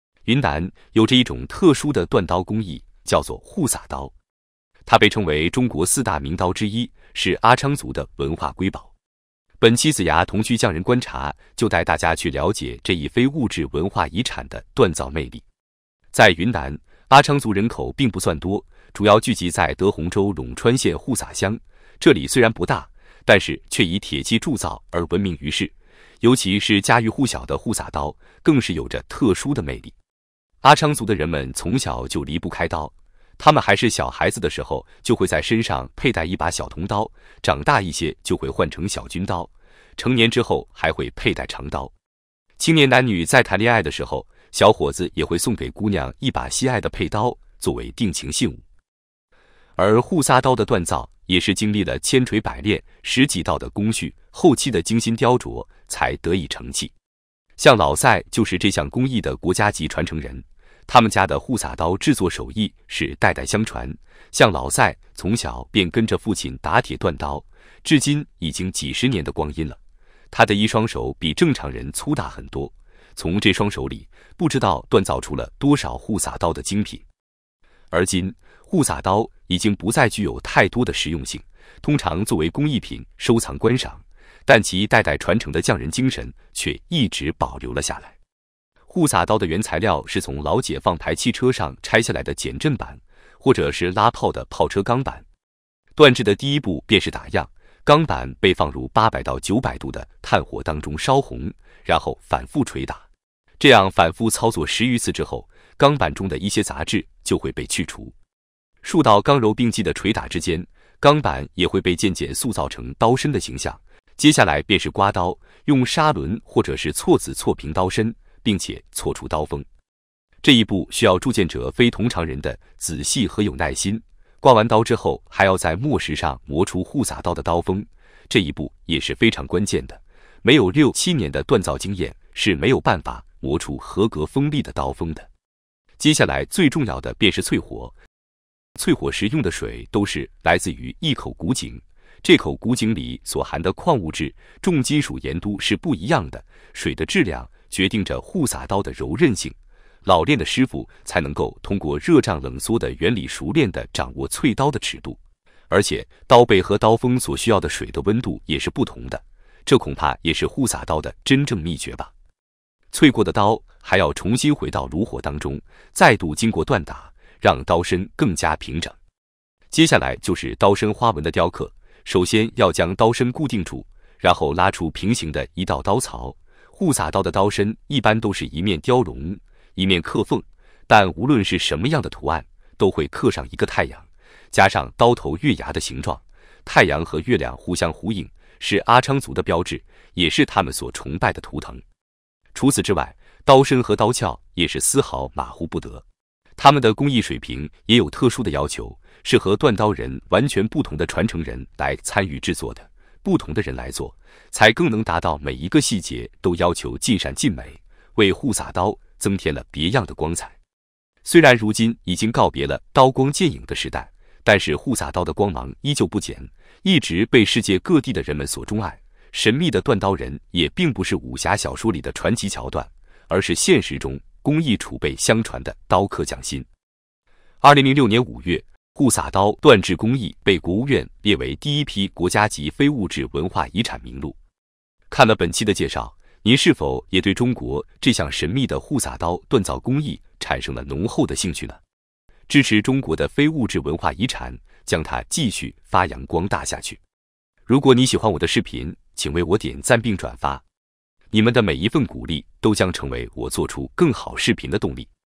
削铁如泥的户撒刀：中国四大名刀之一，千年传承的非遗技艺
在云南，有着一种特殊的锻刀工艺，叫做户撒刀。它被称为中国四大名刀之一，是阿昌族的文化瑰宝。本期子牙童趣匠人观察就带大家去了解这一非物质文化遗产的锻造魅力。